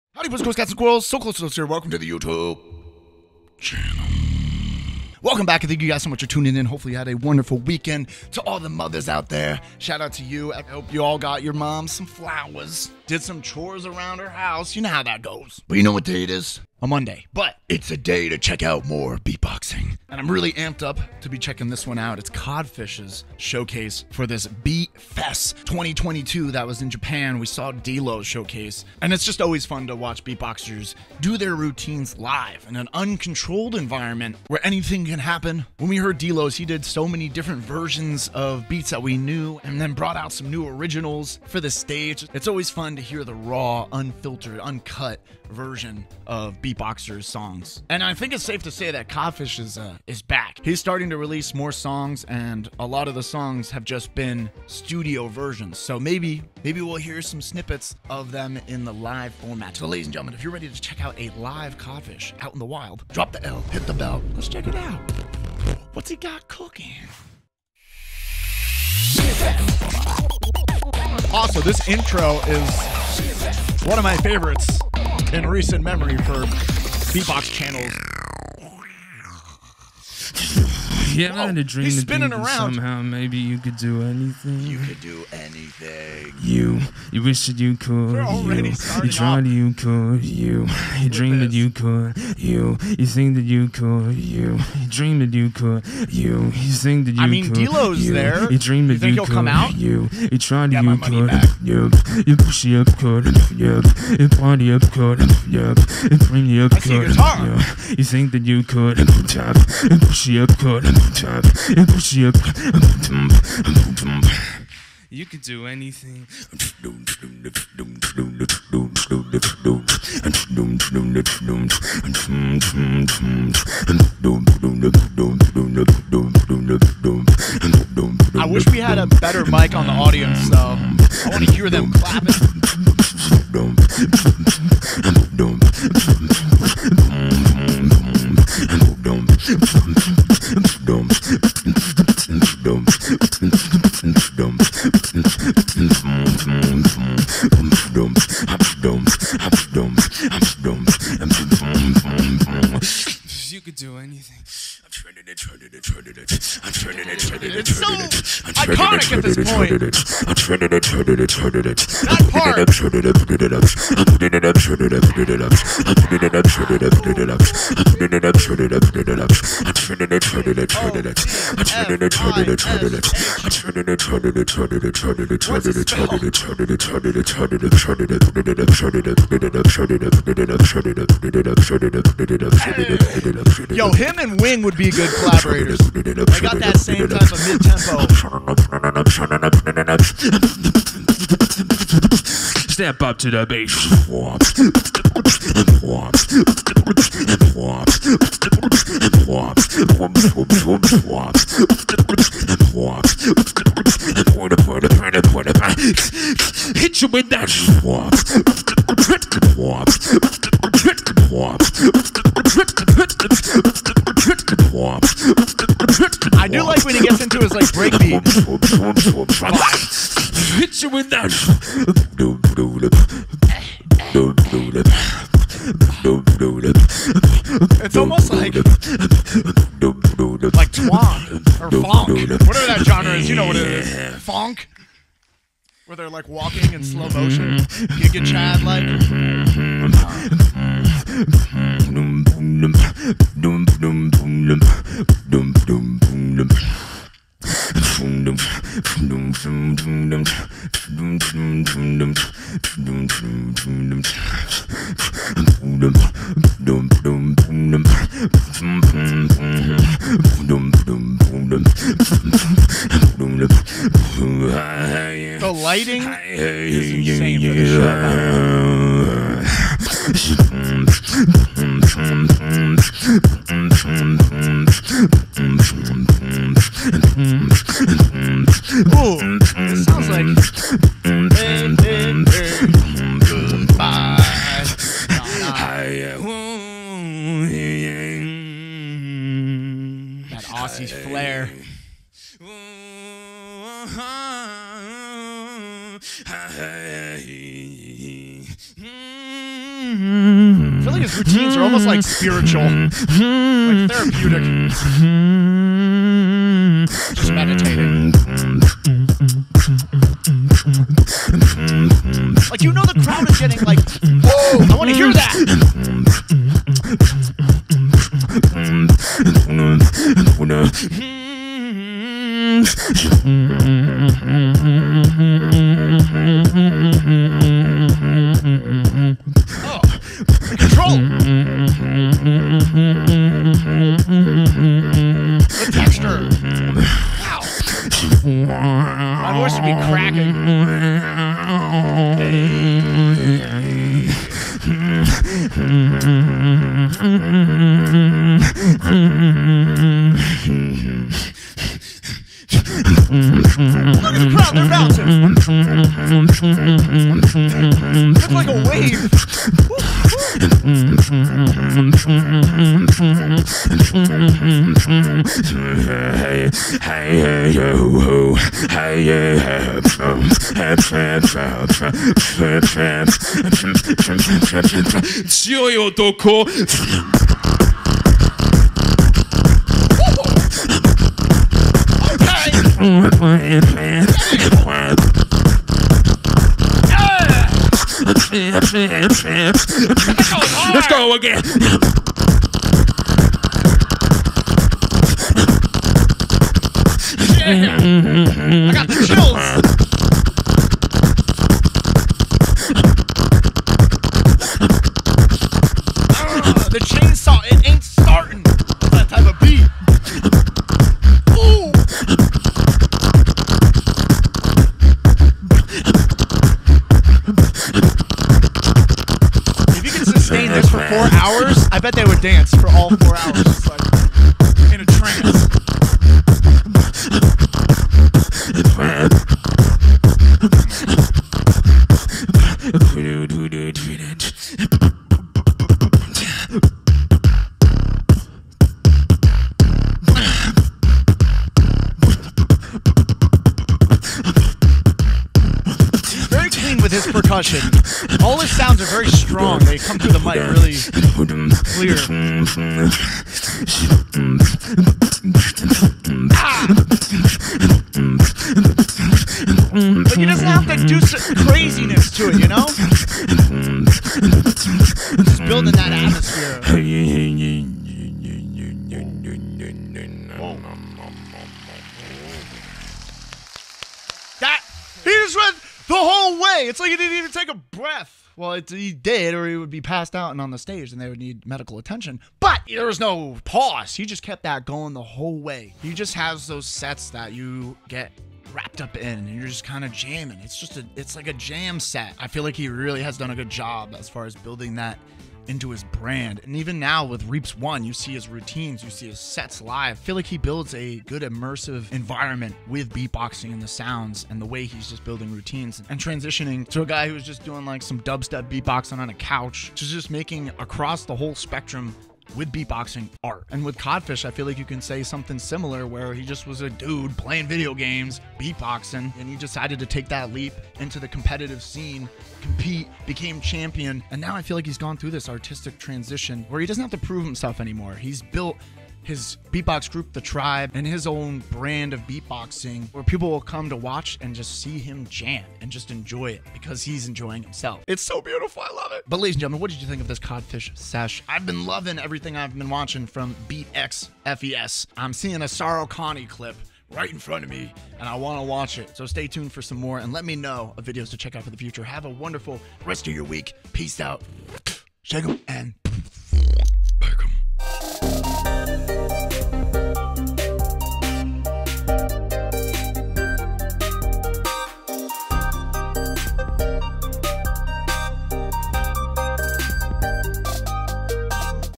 Codfish | BEAT X FES 2022 IN JAPAN BEATBOX REACTION!!!
Codfish is having himself a couple of amazing weeks! We have been blessed by the beats and appreciate hearing from Codfish so much as of late! What did you think of his most recent showcase?!

Thank you for tuning into my reaction videos! Thank you for all the support and love!